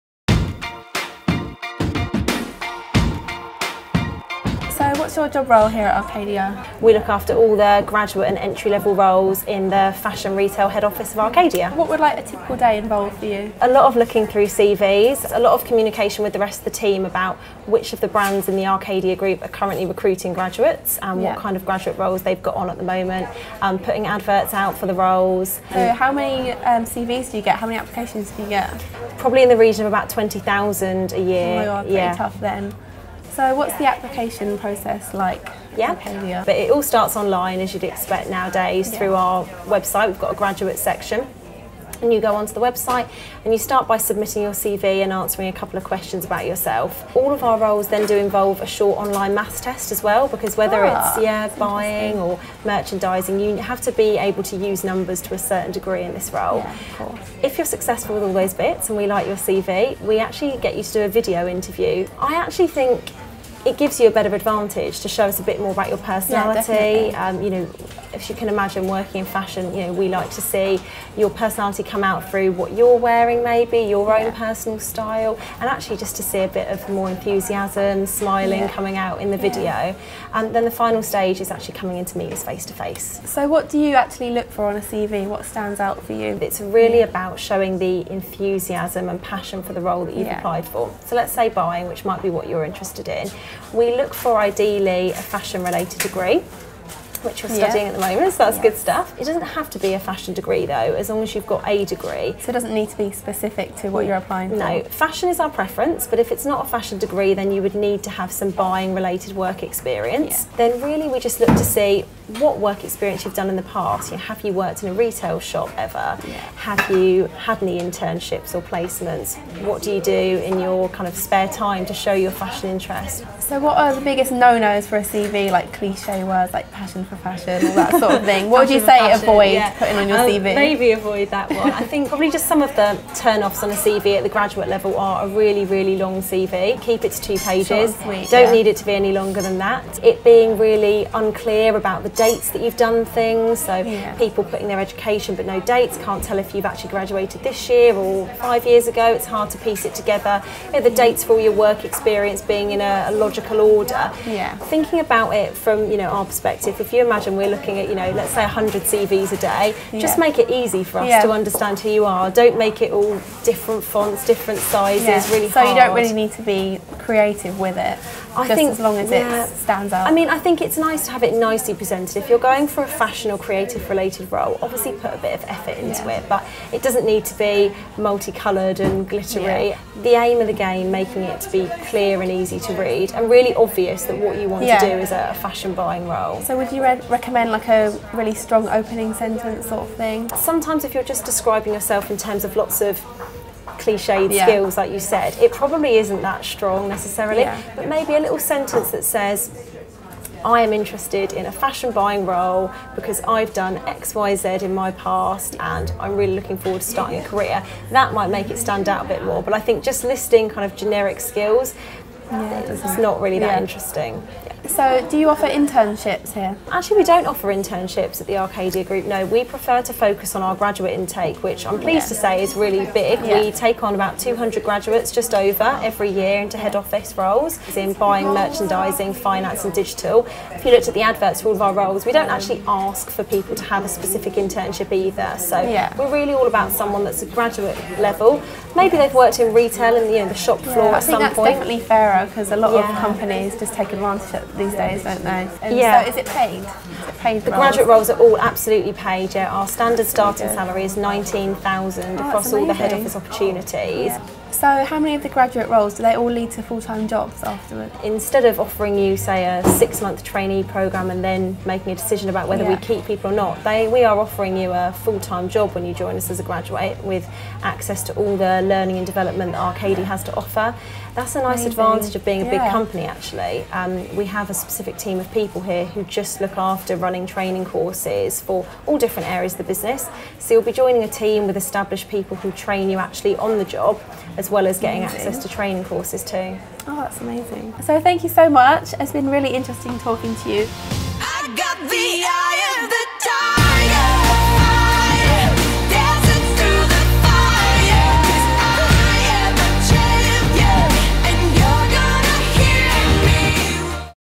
How to start your career with Arcadia Future Talent
Fashion Student and blogger Lucy Rance aka Lulutrixabelle speaks with Rachel Wicks from the Arcadia Future Talent Team.

Clothes Show TV pops into Arcadia HQ for an exclusive chat with Rachel Wicks from the Arcadia Future Talent Scheme. Rachel tells us all about the Arcadia recruitment process and offers advice on how to avoid CV blunders and also how to stand out from the crowd.

Keep checking back for more fashion advice, exclusives and trends.